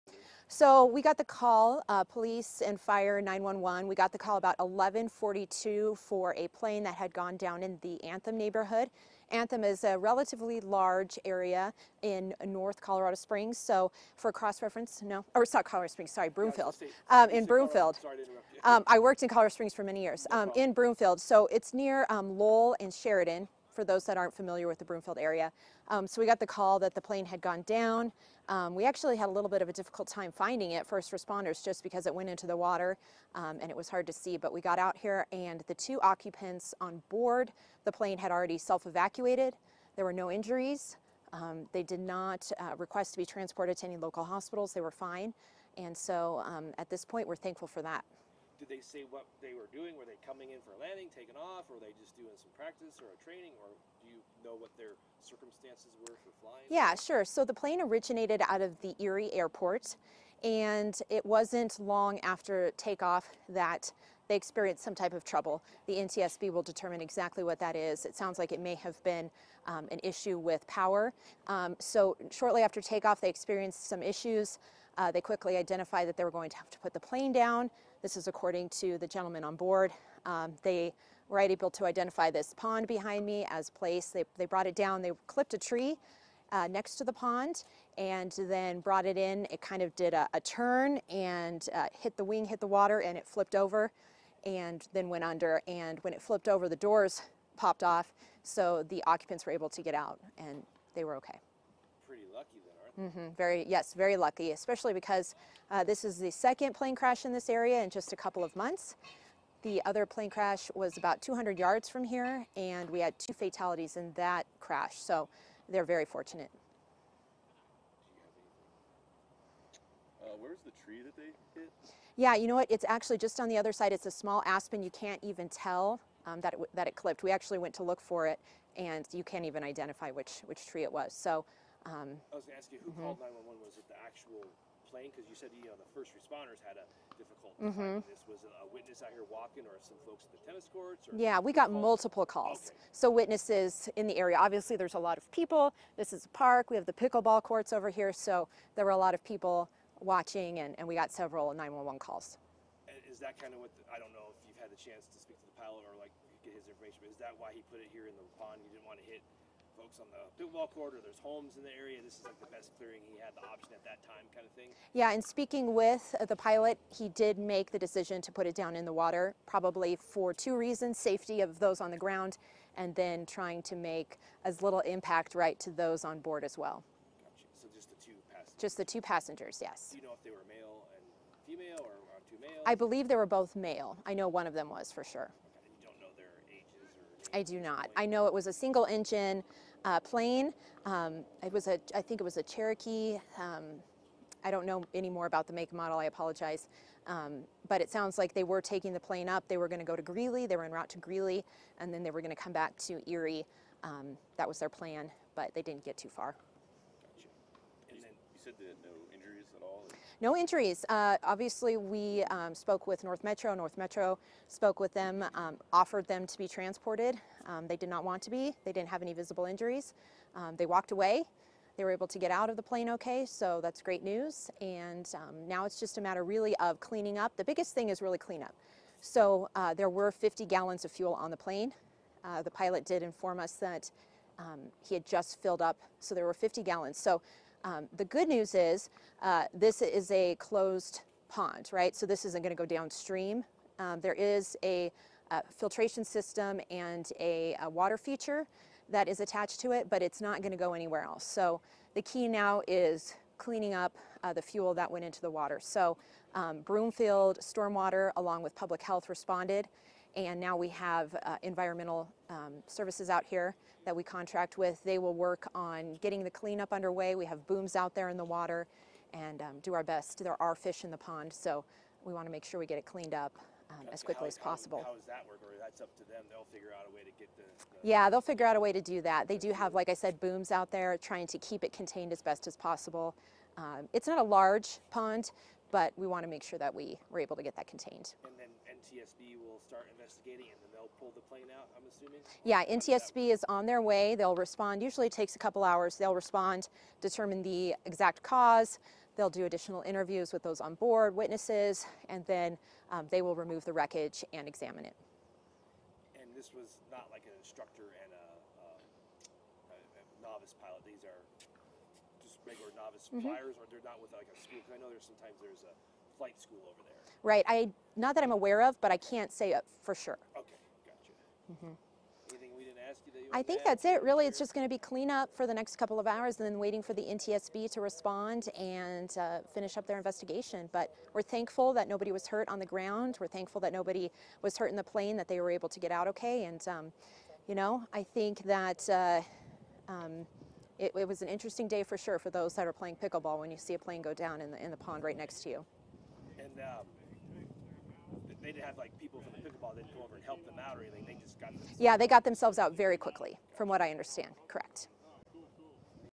2 men walked away from Broomfield plane crash
A single-engine plane crashed in a pond of a subdivision in Broomfield but the two occupants were not injured.